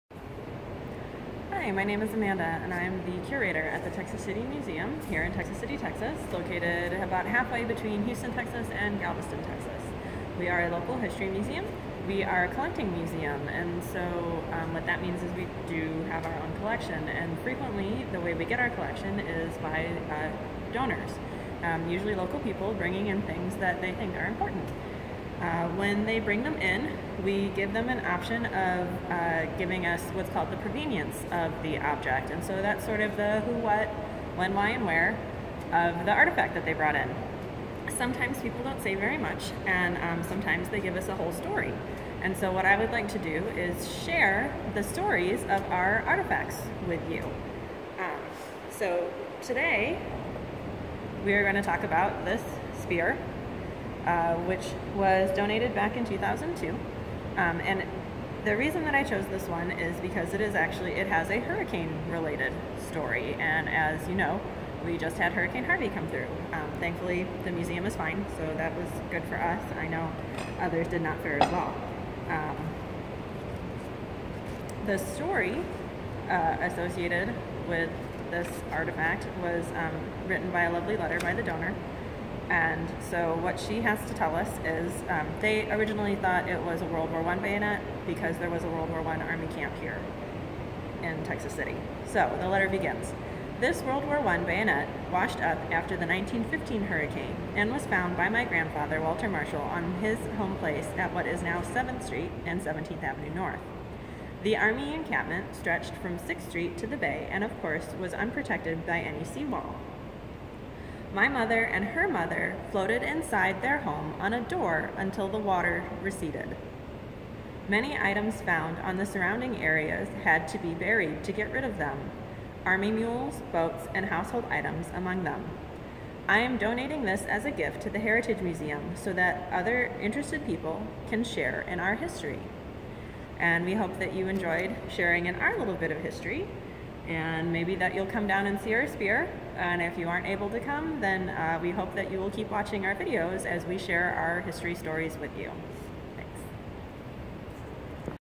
Tales from the Texas City Museum Episode 1
These videos focus on the collection of the Texas City Museum and the stories associated with the objects. This video is on a spear found in the 1915 hurricane.